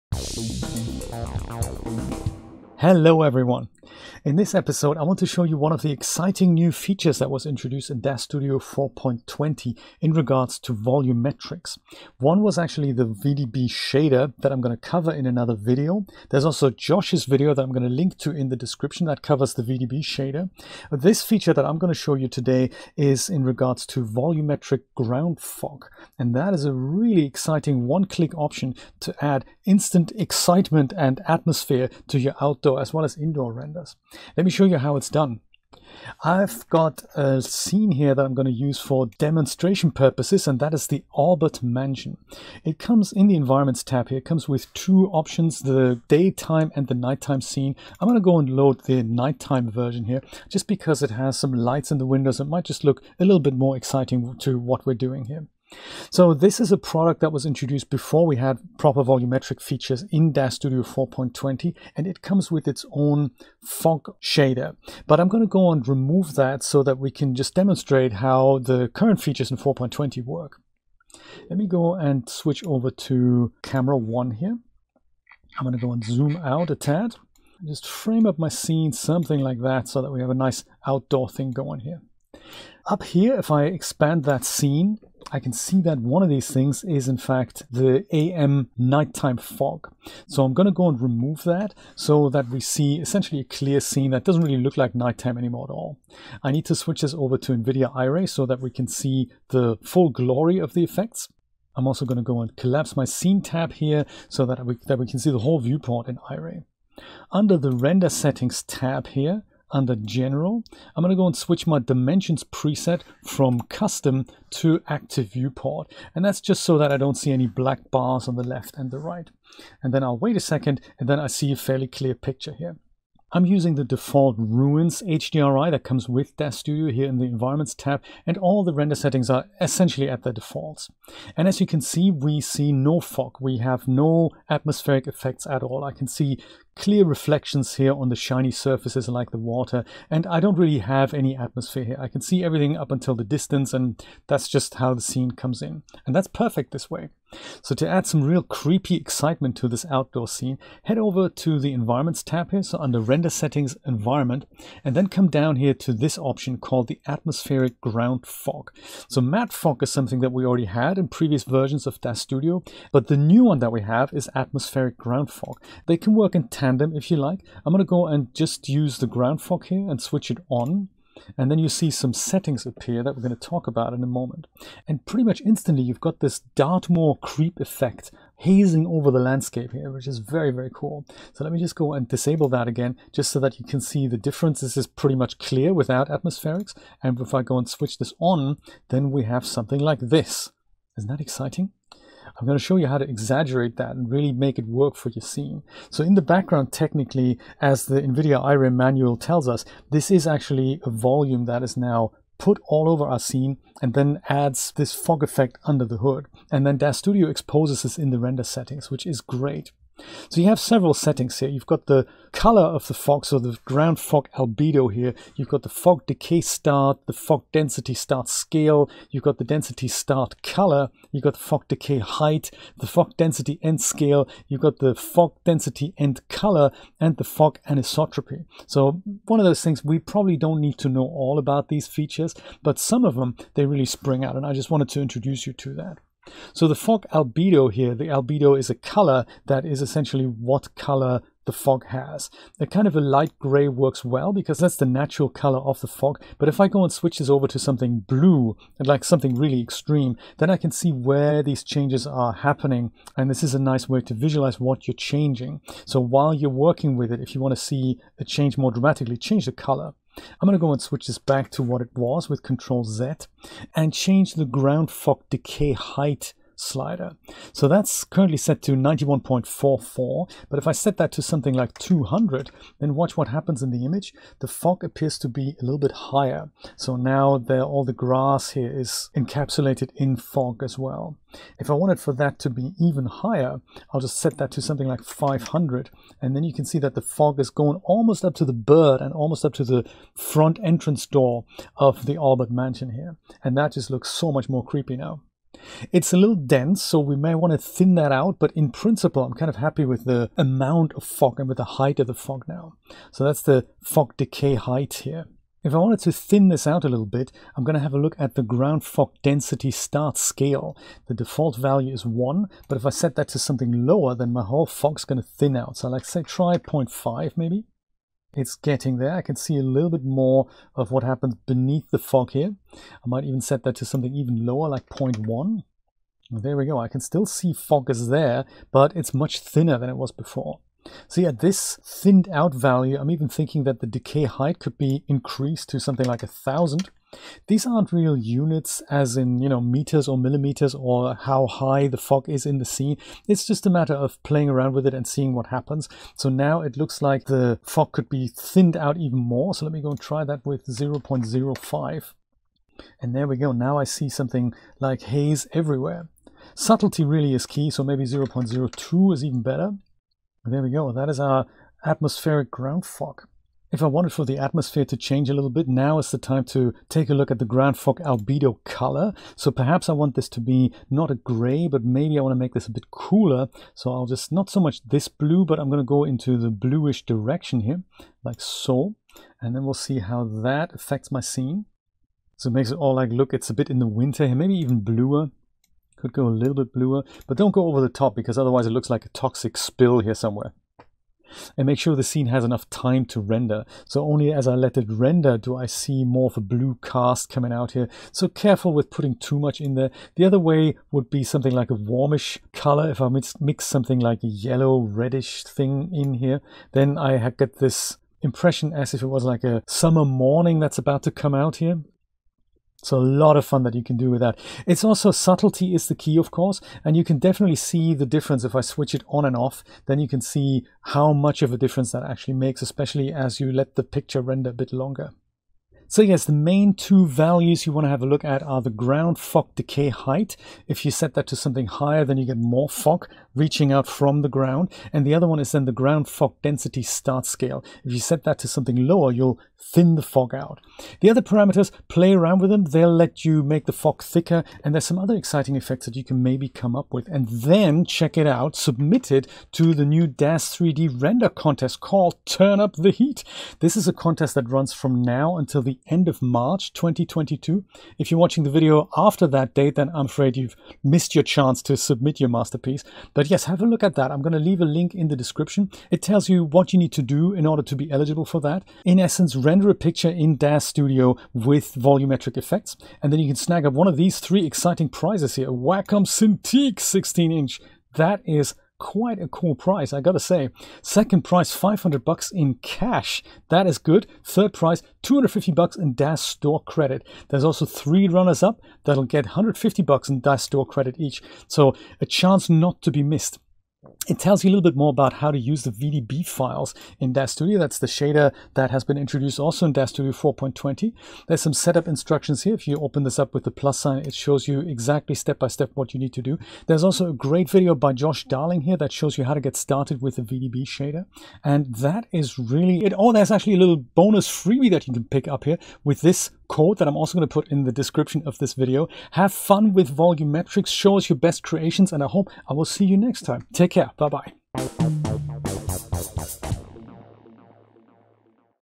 Creating Atmospheric Ground Fog with Volumetric Effects in Daz Studio
In this quick tip, Jay shows you how to set up Atmospheric Ground Fog in your scenes. Use the new volumetric features in Daz Studio 4.20 to add realistic fog effects to your environments in a single click.

Turn Up The Heat and enter our Volumetrics Render Contest (ends March 31, 2022):

Create your own renders and animations